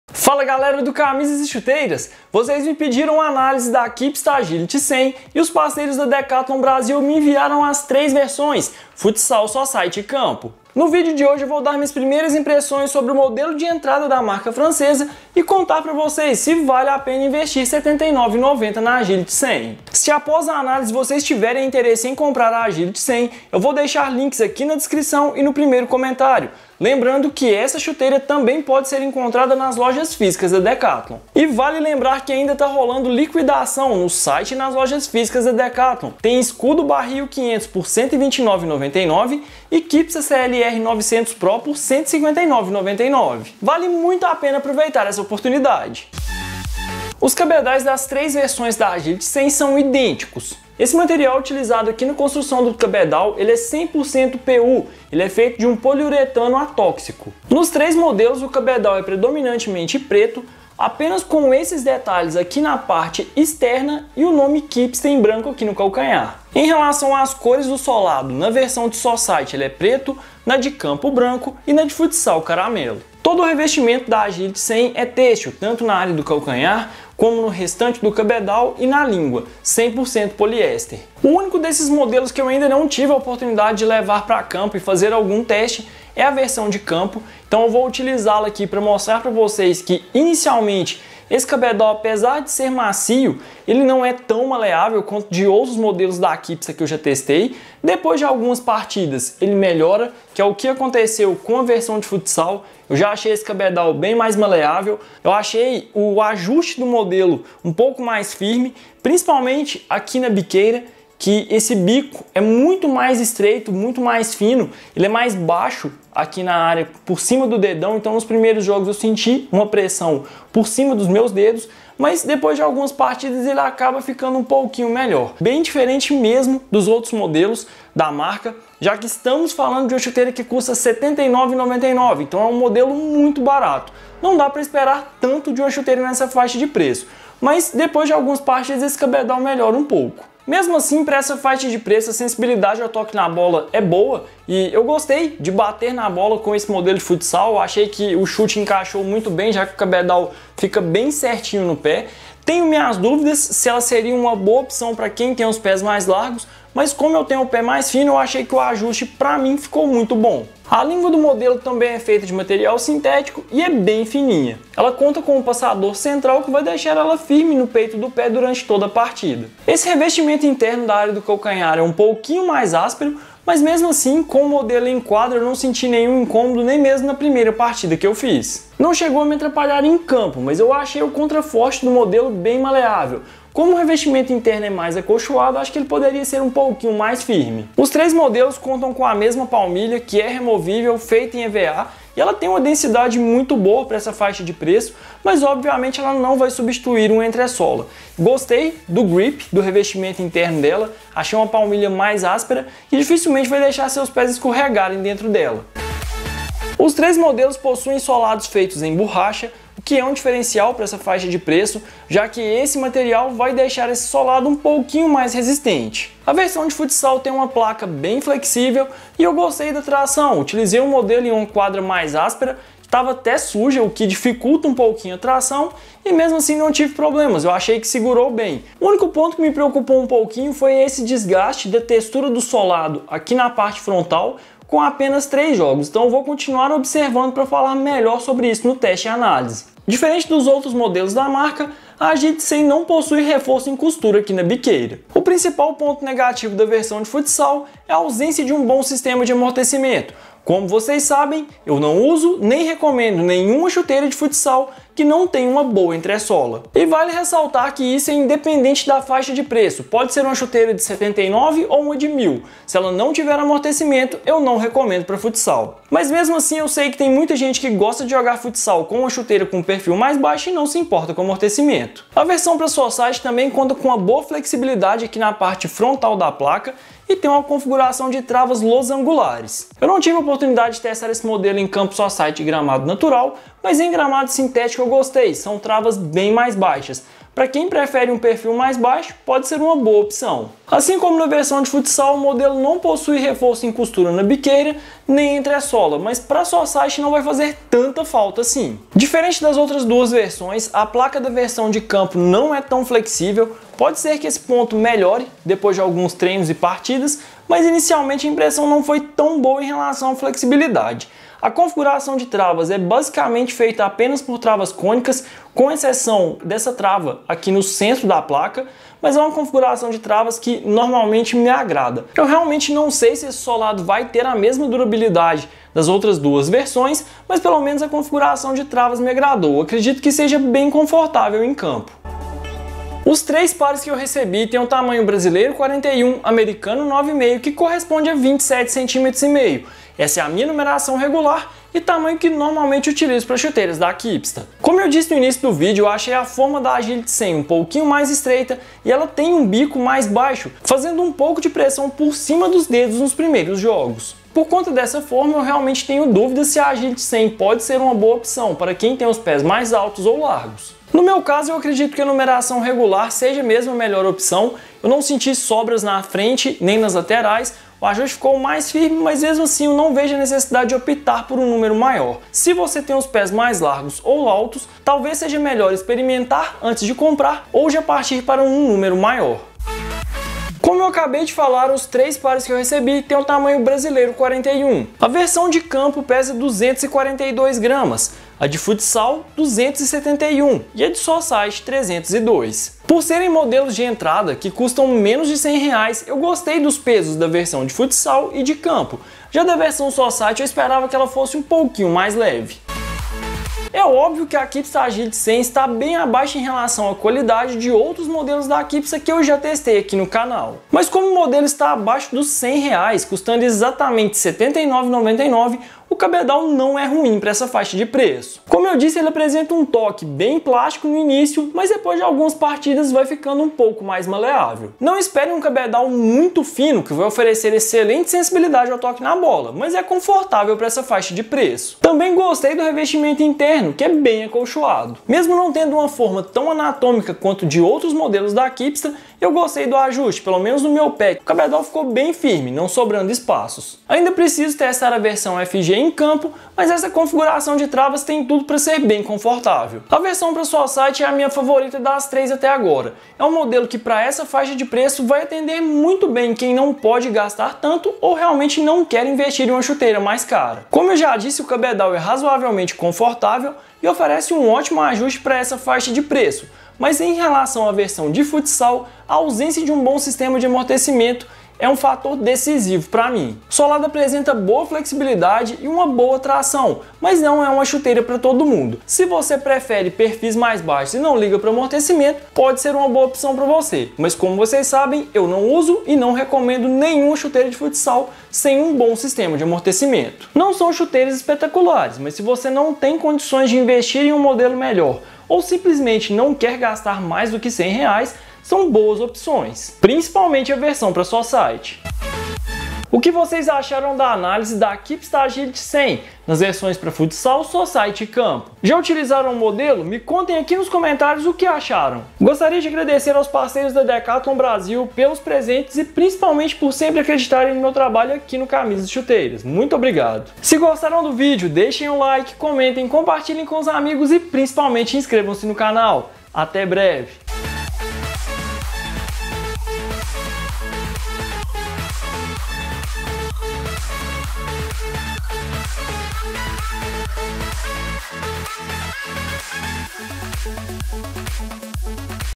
Chuteira BOA e BARATA por R$ 79,90? - Análise chuteira Kipsta Agility 100 Futsal, Society e Campo
Fala galera do Camisas e Chuteiras! No vídeo de hoje eu analisei as chuteiras mais baratas da Kipsta. Será que vale a pena investir na Kipsta Agility 100 campo, society e futsal?